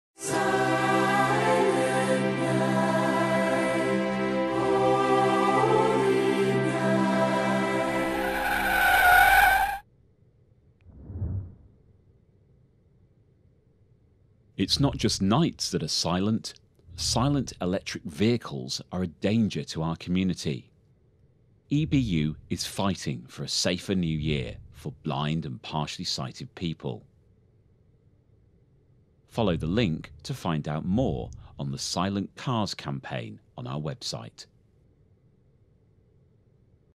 EBU xmas video
EBU Christmas / New Year Video using the theme to highlight out silent cars warning campaign